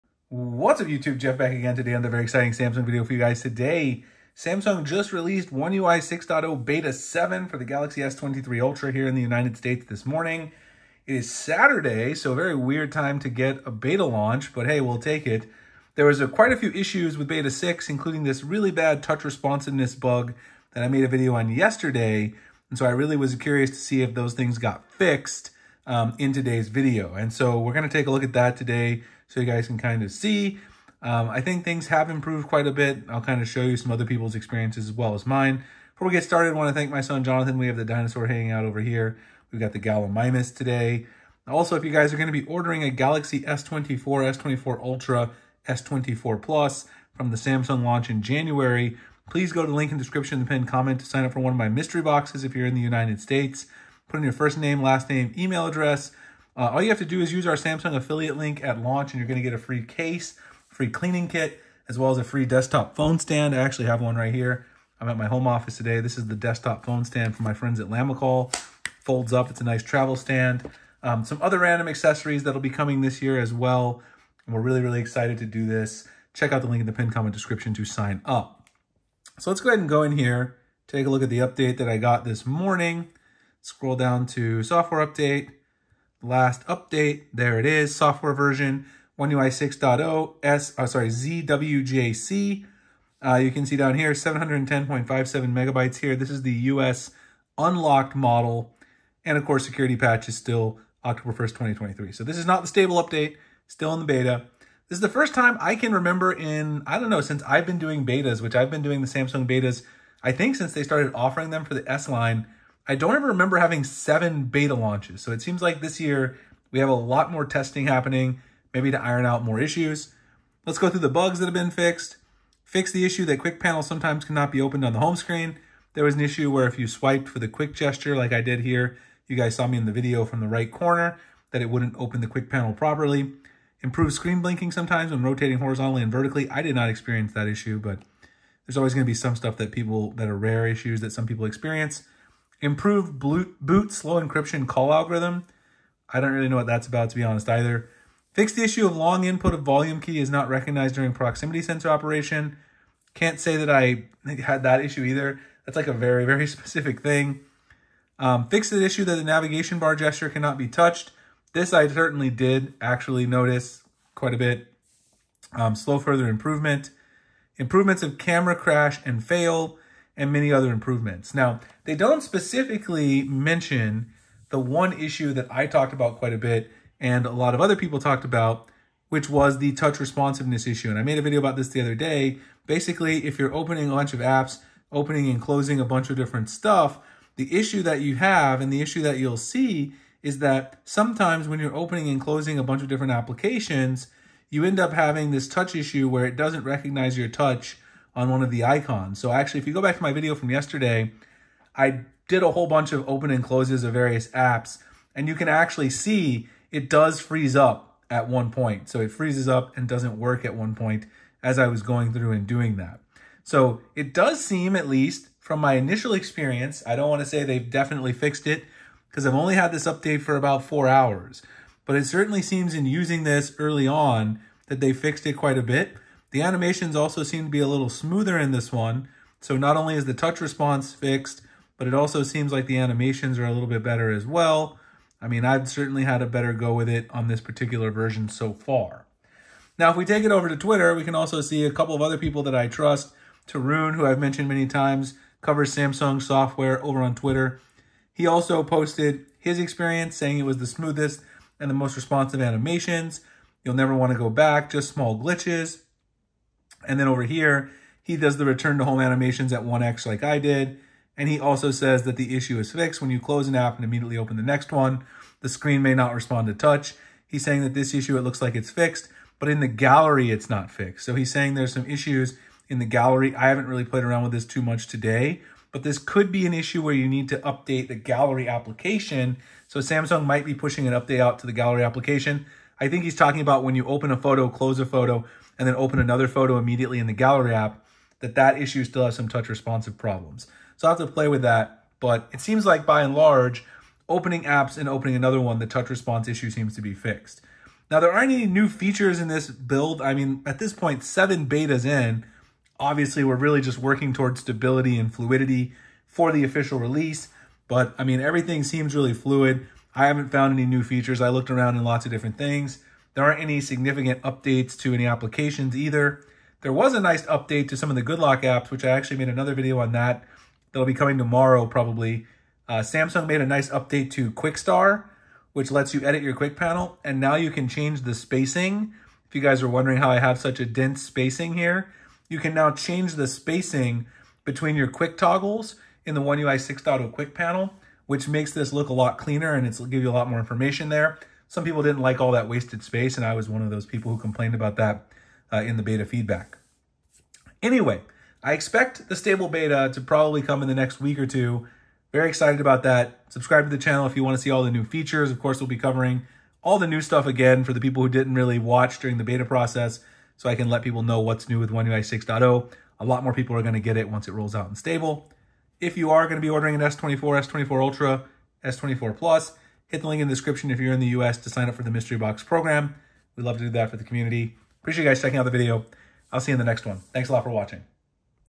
Samsung One UI 6.0 Beta 7 is OUT - All New Features and Changes (Animations Fixed)
Check out my partner Mint Mobile to get awesome Unlimited Premium Wireless for $15 for a limited time